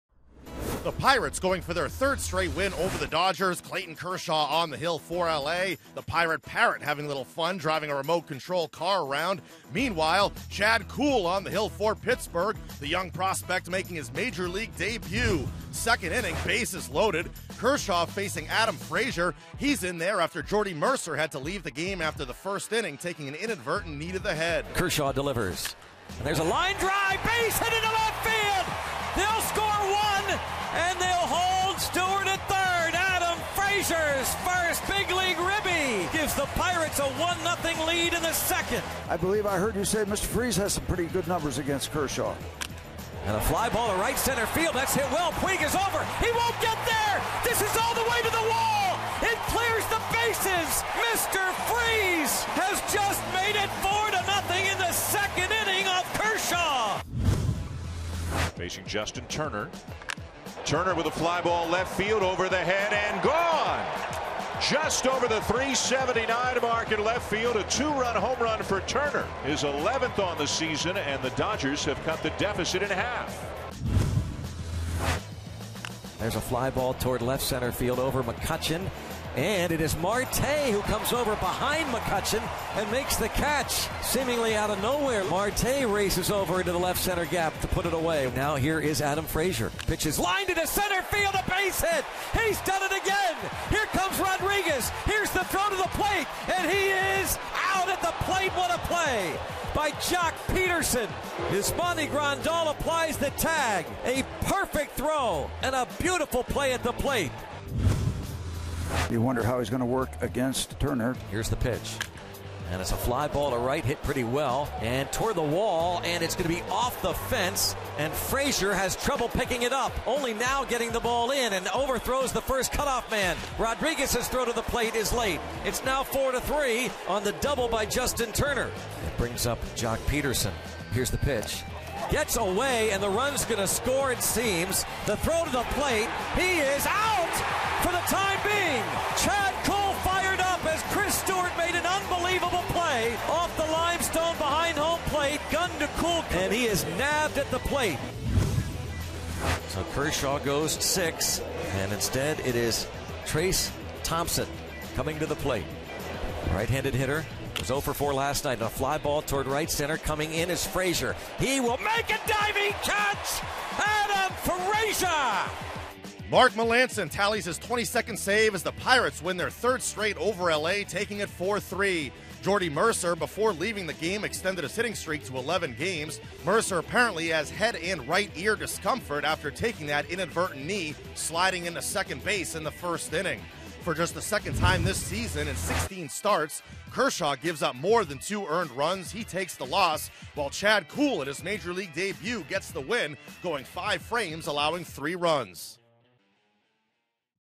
6/26/16: Kuhl outduels Kershaw to tally first MLB win
Daily Recap: Chad Kuhl allowed just four hits over five and the Pirates put up four runs against Clayton Kershaw to win their third straight